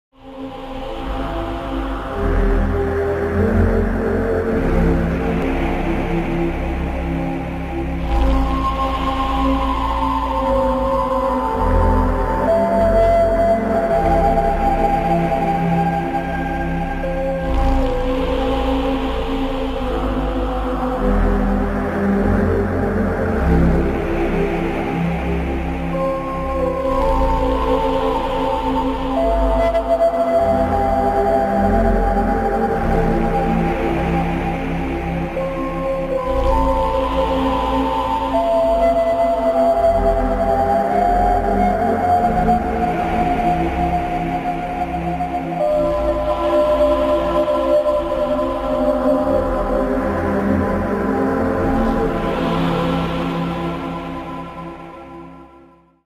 Speed Oil Painting | Live on Instagram #1
Timelapse/Speed Oil Painting with Rembrandt Oil Color on hardboard. It was a spontaneous live video on instagram.

Hardboard 40 x 50cm, Oilpaint

1 Hour Work